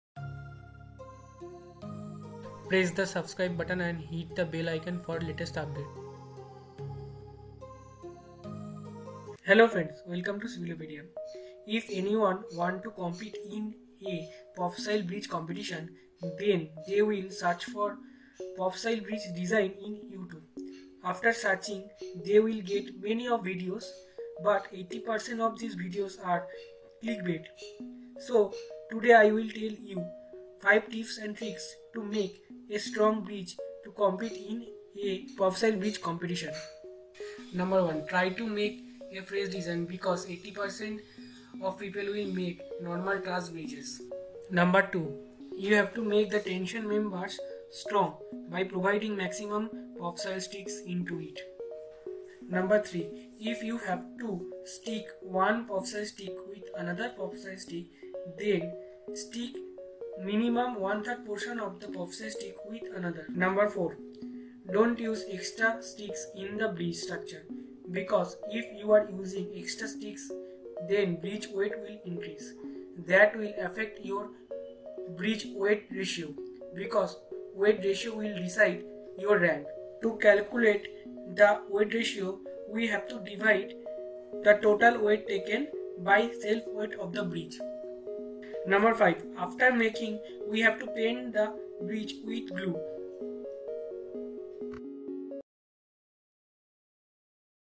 how to make a strong popsicle bridge | 5 ticks and tips for popsicle bridge compition
follow this tips and make strong bridge to win a popsicle bridge making competition. best of luck